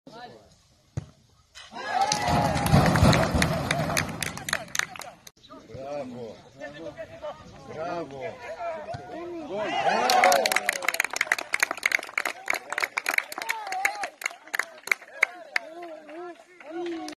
ΠΑΟ Κάτω Μηλιάς - Ακαδημία Κατερίνης 2006 1-2: Τα γκολ των φιλοξενούμενων
Δείτε τα δύο γκολ της Ακαδημίας Κατερίνης 2006 από την φιλική νίκη με 2-1 επί του ΠΑΟ Κάτω Μηλιάς, σε αναμέτρηση που διεξήχθη στο γήπεδο της Κάτω Μηλιάς, το απόγευμα της Κυριακής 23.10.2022.

Σκόρερ και των δύο γκολ ο Παρασίδης, στο 60' και στο 80'.